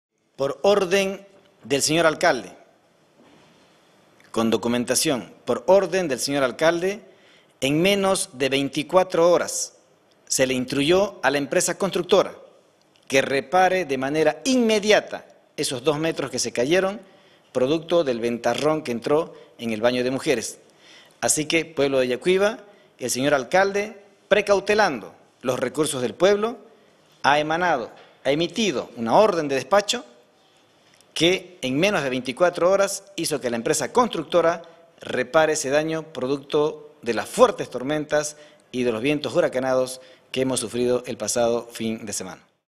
POR ORDEN DEL ALCALDE VALLEJOS, EMPRESA CONSTRUCTORA REFACCIONA CIELO FALSO
ALCALDE INSTRUYE A EMPRESA REPARAR DE MANERA INMEDIATA LOS 2  METROS DE CIELO RAZO DEL BAÑO DEL MERCADO CENTRAL; CONSECUENCIA DEL AGUA INGRESADA POR UNA VENTANA ABIERTA  Y  EN  MENOS DE 24 HRS SE DA SOLUCIÓN